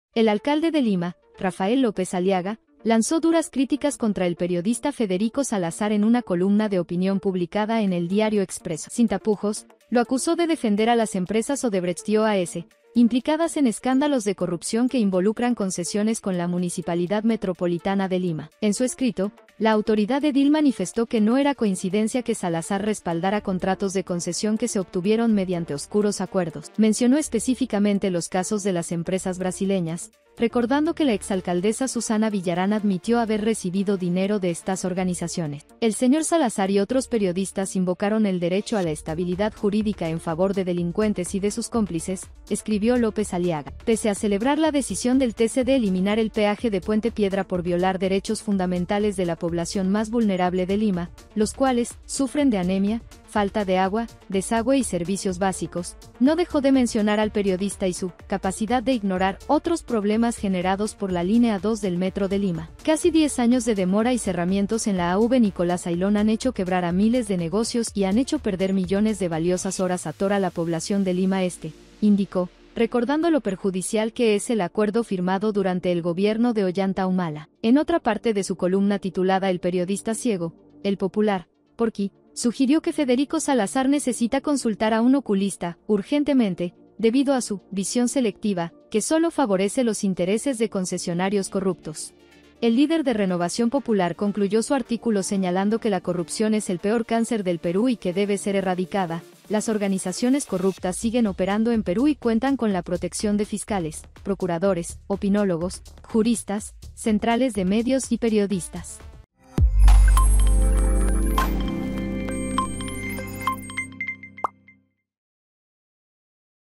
R. López Aliaga y sus punzantes frases a Federico Salazar por "defender a corruptas Odebrecht y OAS"
Rafael López Aliaga y sus punzantes frases a Federico Salazar por “defender a corruptas Odebrecht y OAS”
"La corrupción es el peor cáncer del Perú y debe ser erradicada", sostuvo alcalde de Lima.